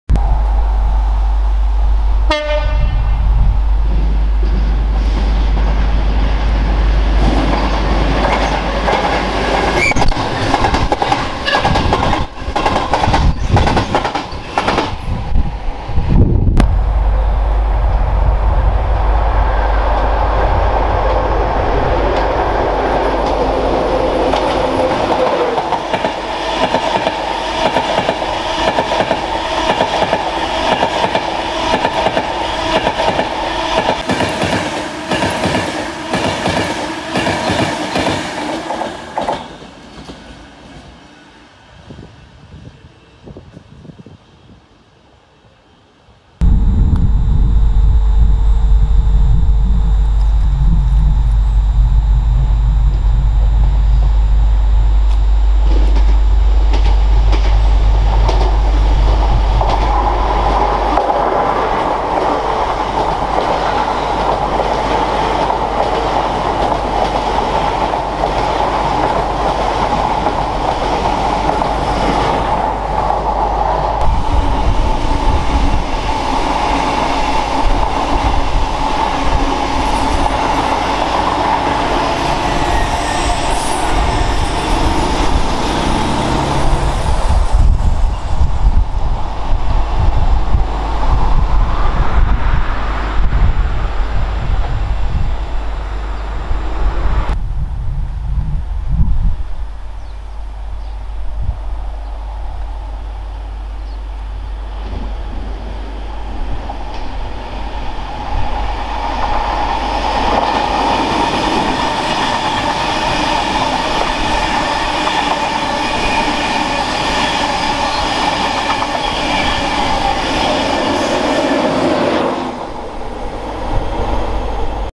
Last Class 91 North of York - Monday 7th September
3 days after the 'last ever' class 91 North of York, makes it's way North from, after issues at London Kings Cross.

Forming the 1s04 Peterborough to Edinburgh and 1e14 Edinburgh to London Kings Cross, a last minute alteration sees 91124 makes its way through Morpeth at 1056 and 1311.

Also seen are a move of 6 class 66s from Millerhill to Leeds Balm Road